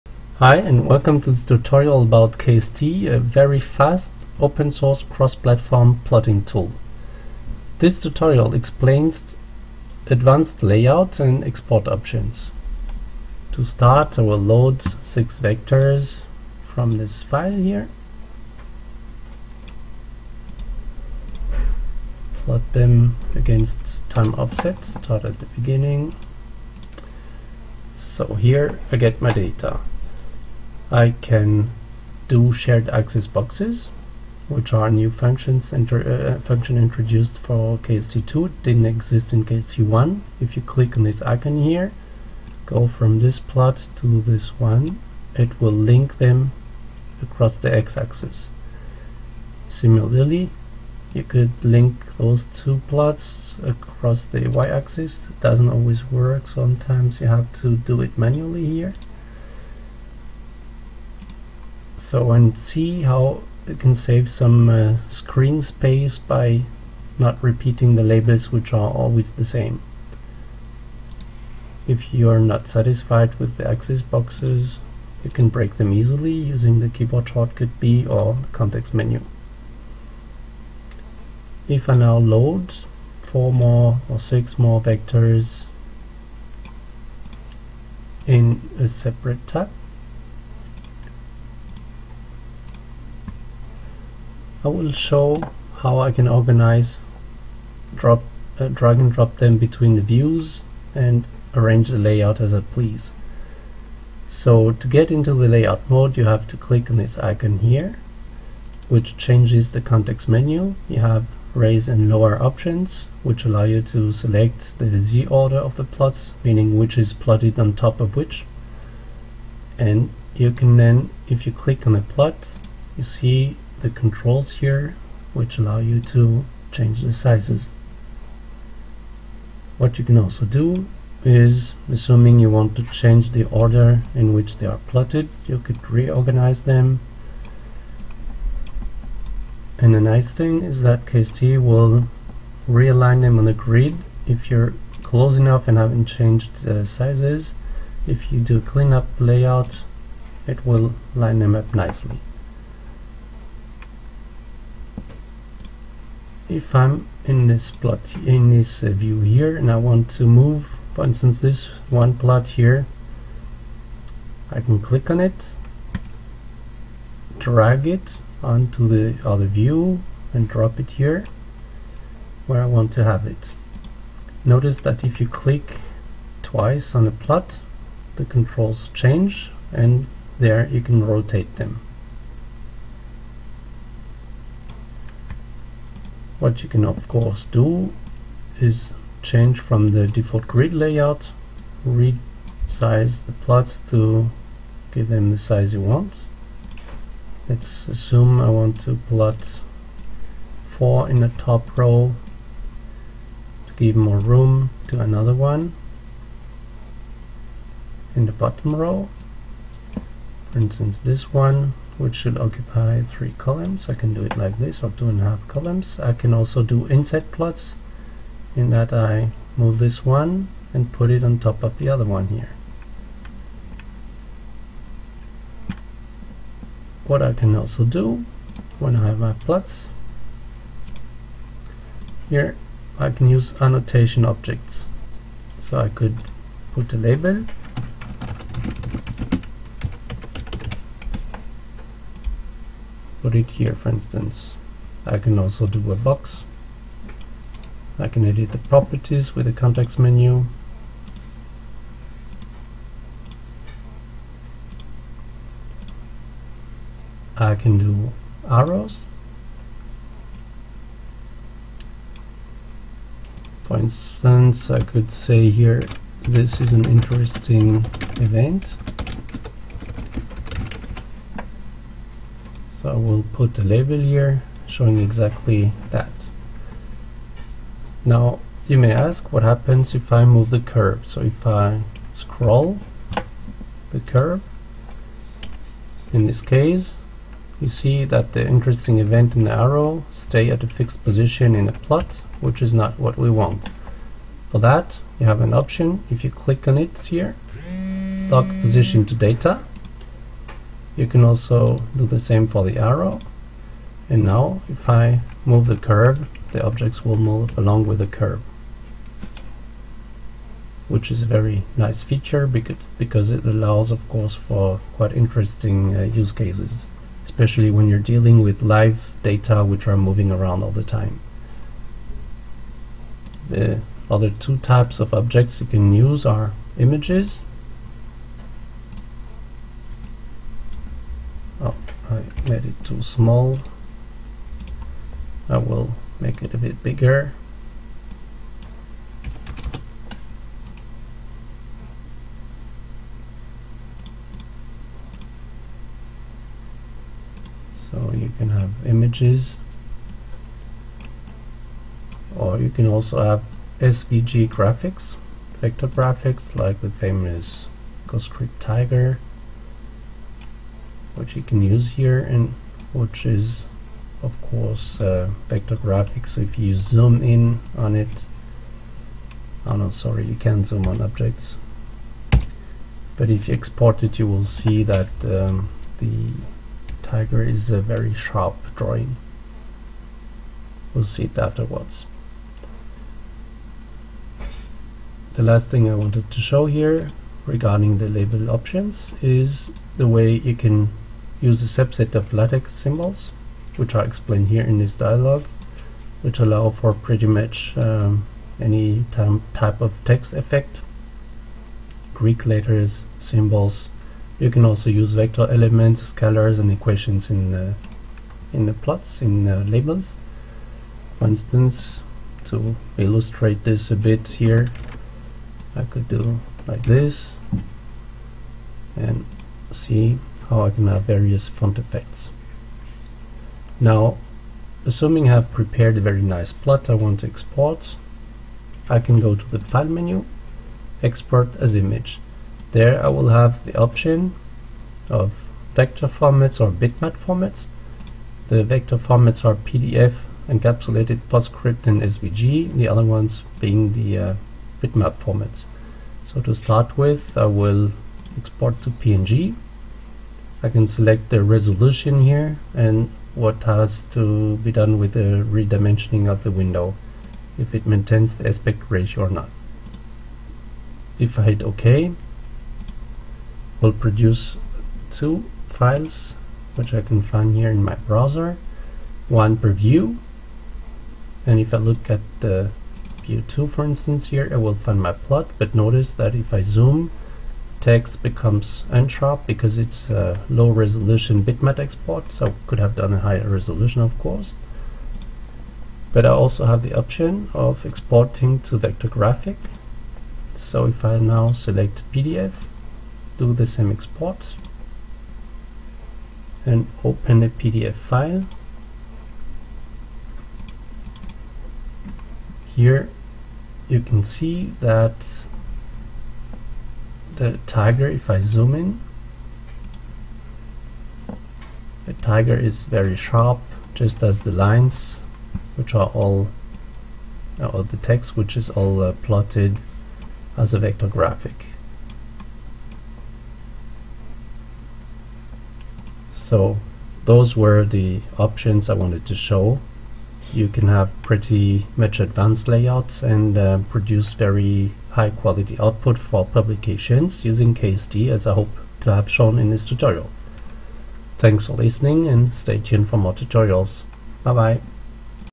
Kst presentation #6: Advanced layout and export options
This tutorial explains advanced layout and export options
- Load some vectors from a file using the Data Wizard
- Show "shared axis boxes"
- Load another few vectors into a separate tab
- Advanced layout mode and inter-tab drag&drop, including inset plots
- Show annotation objects including SVG and image objects
- Show the "Lock position to data" feature
- Advanced label options (Latex subset) and font effects
- Export possibilities to either bitmap or vector formats

For more information on Kst visit the homepage and watch the other videos on this channel.